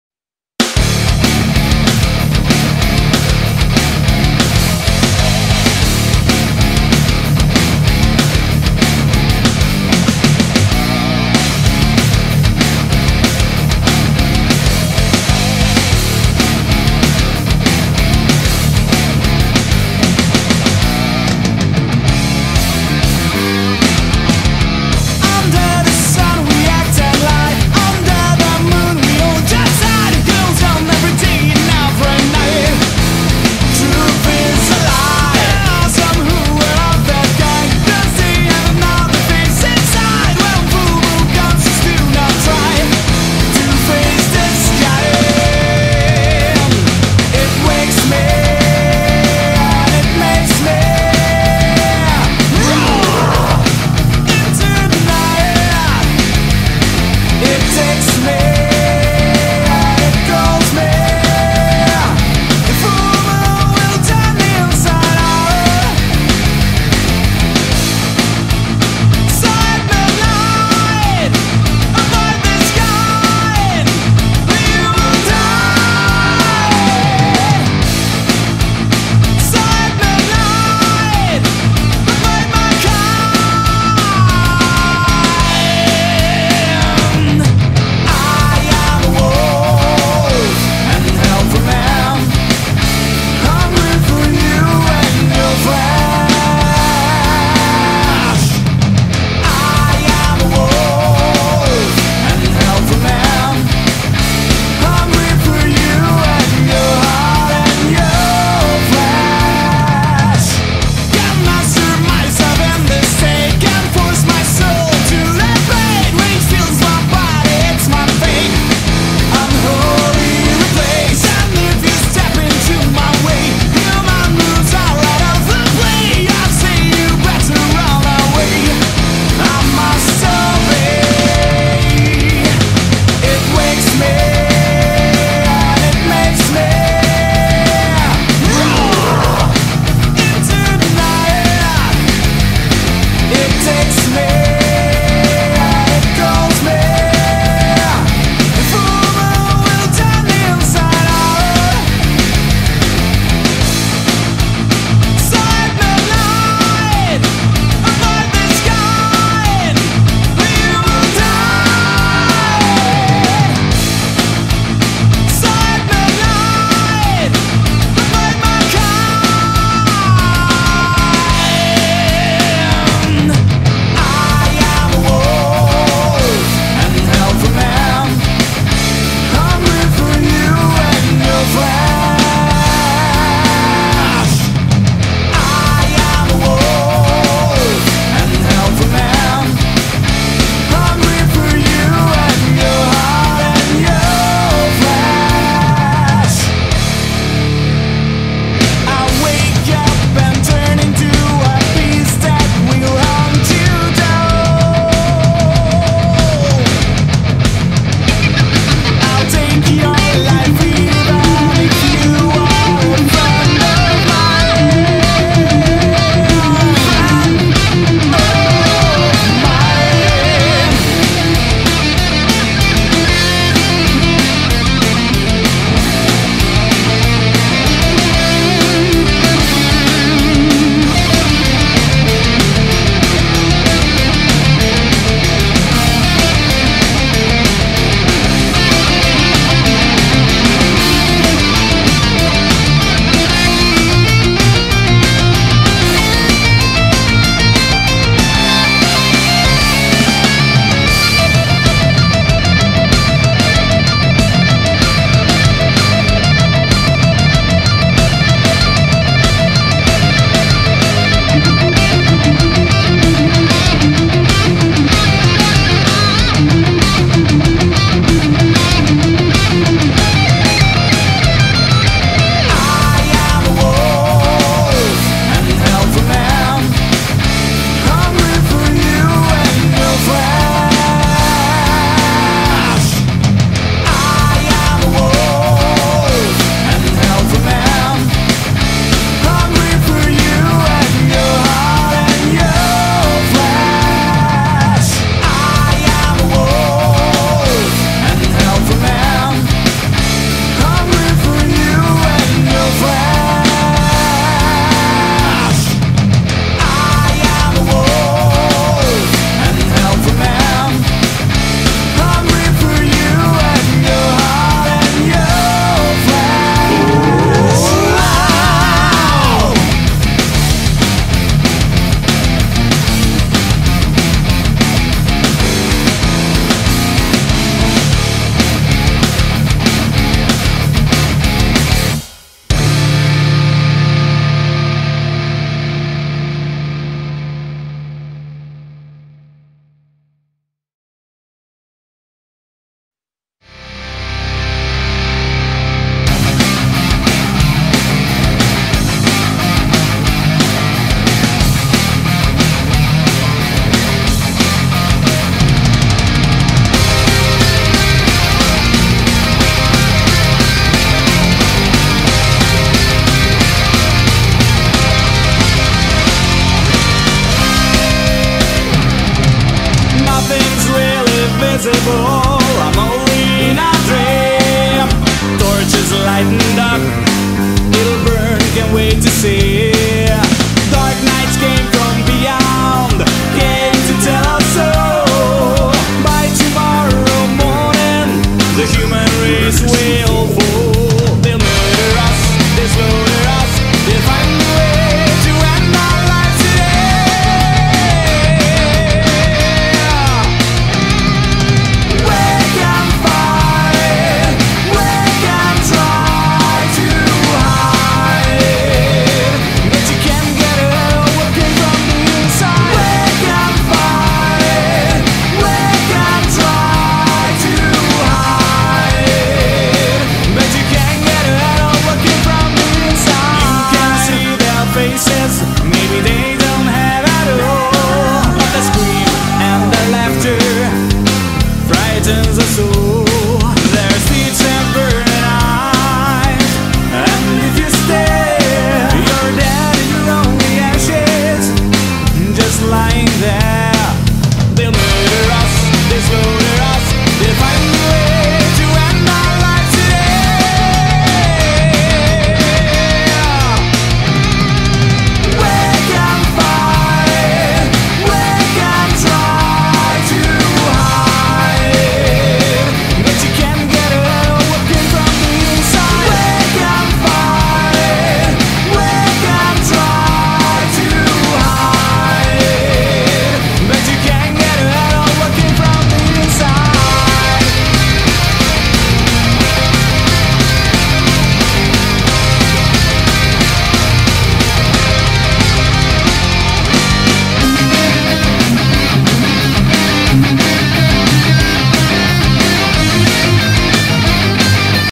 Pogno hd sony ps2
Video girato dai "Senza sella team" a Pogno il 18/11/2012 con la Sony actioncam